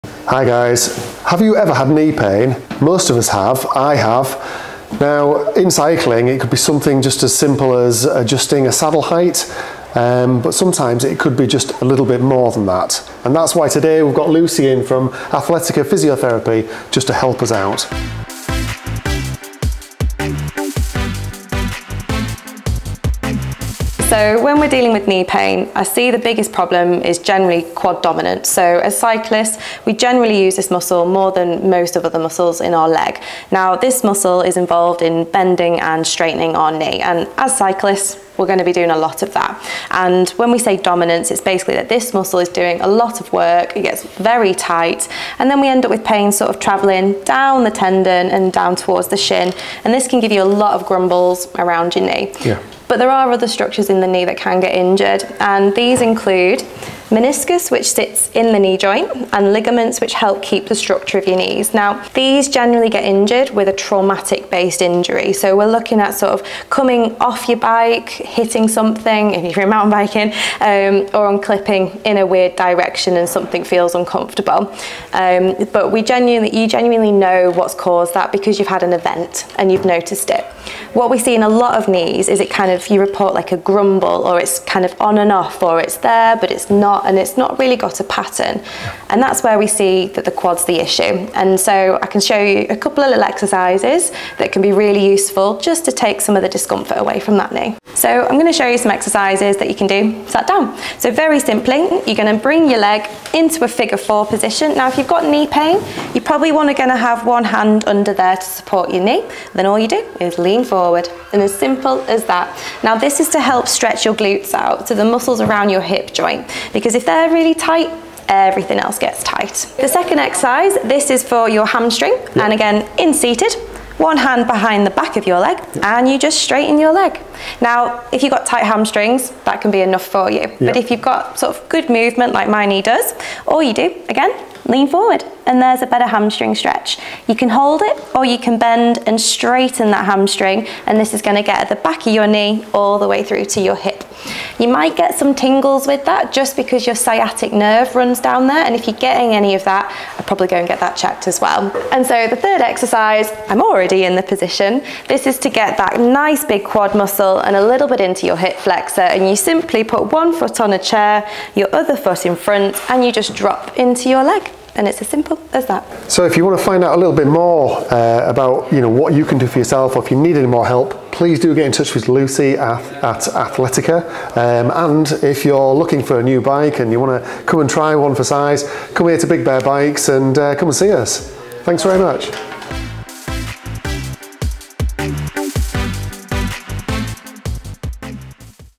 3 Simple Knee Pain Exercises For Cyclists
Lucy from Athletica Physiotherapy demonstrates three essential exercises to alleviate knee pain for cyclists. In this video, she presents simple, equipment-free stretches that can be performed at home to help maintain knee health and prevent discomfort.

In this video, we cover three exercises:
• Glute Stretch: A seated stretch to relieve tightness in the glutes and hip area.
• Hamstring Stretch: An exercise to improve flexibility in the hamstrings and alleviate tension in the back of the knee.
• Quad and Hip Flexor Stretch: A movement to stretch the quadriceps and hip flexors, helping reduce pain caused by quad dominance.